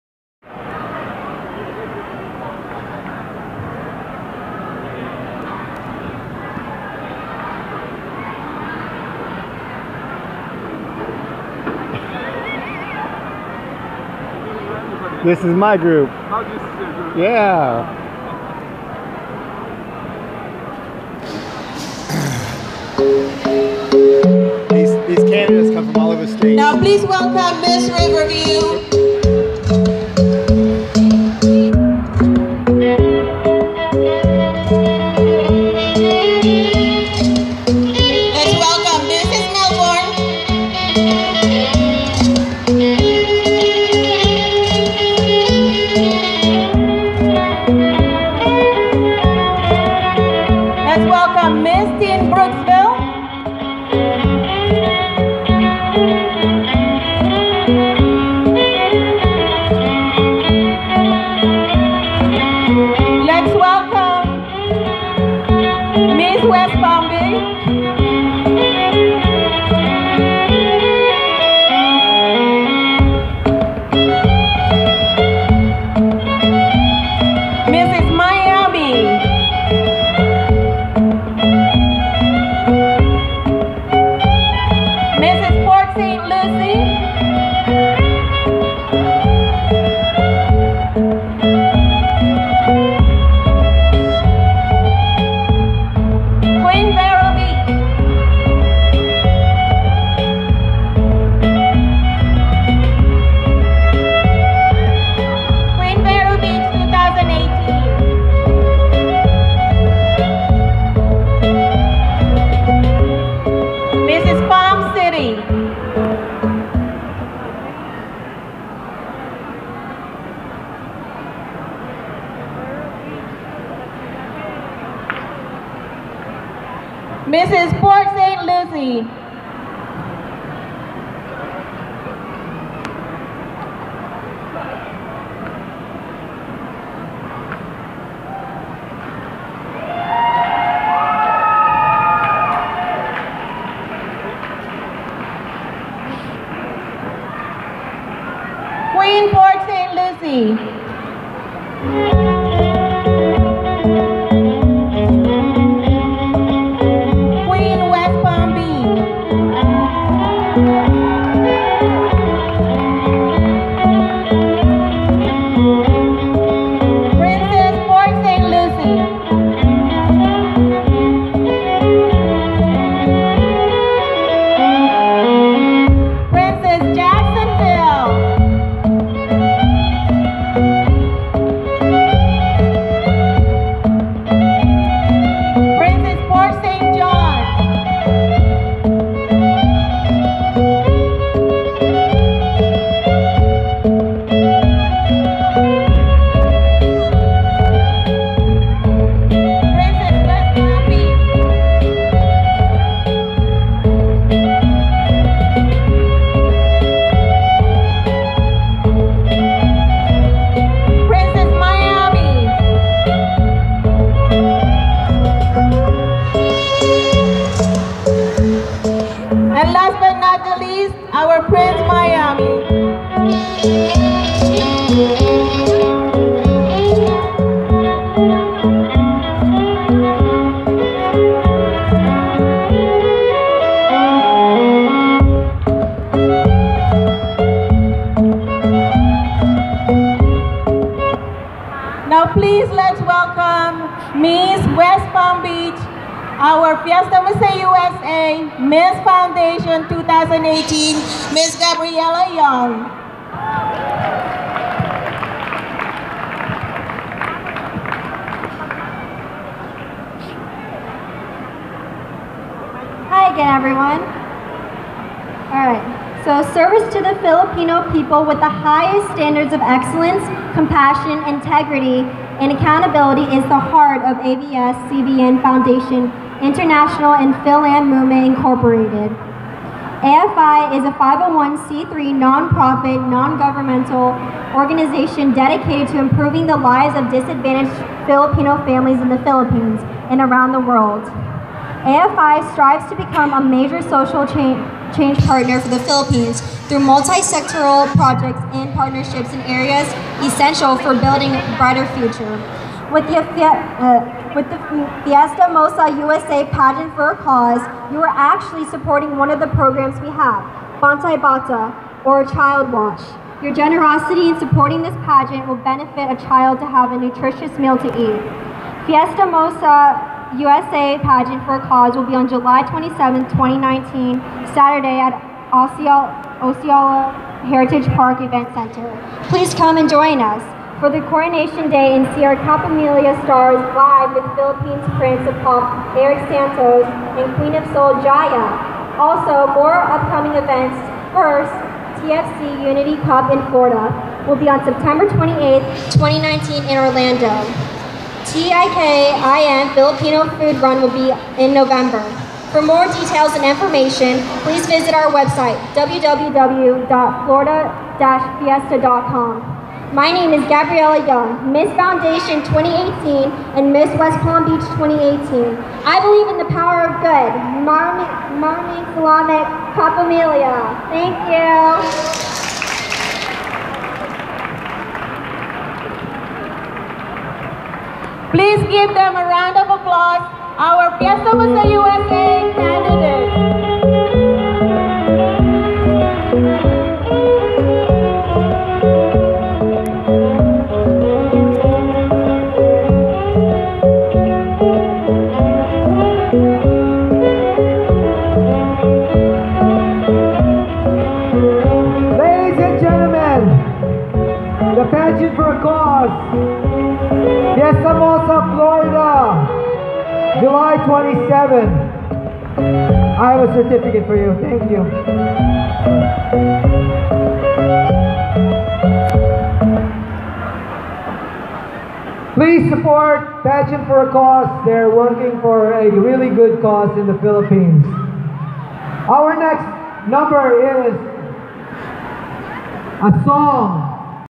Video of Professional Vogger Jimbo360 (AKA: Napkin Wizard) at the Summer Filipino Festival in FL 📹
This event is held in West Palm Beach, Florida it is a Summer Festival in the Filipino community and celebrates Independence Day in the Philippines, I caught Jimbo360  vlogging the Fiesta Mo Sa  Florida beauty contestants while visiting the event. 😅